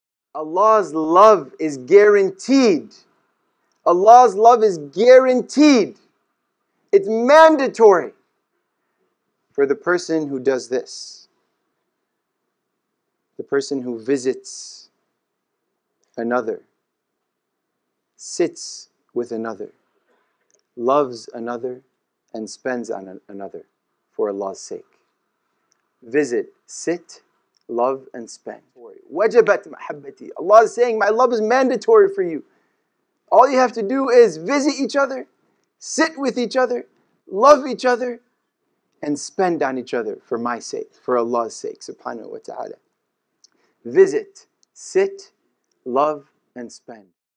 Allah’s Love is Mandatory For This Person | Sidi Mahdy Amine
Sidi Mahdy Amine shares comforting spiritual insights about the guaranteed love for the person who visits, sits, loves, and spends on another person for the sake of God.

This talk was delivered at the Muslim Community Center - East Bay (MCC East Bay) in Pleasanton, California on May 2, 2022.

Mahdy Amine is a Zaytuna College alumnus and a practicing attorney. He also teaches Islamic Studies at ILM Tree and he serves as Convert Care Coordinator for Ta'leef Collective.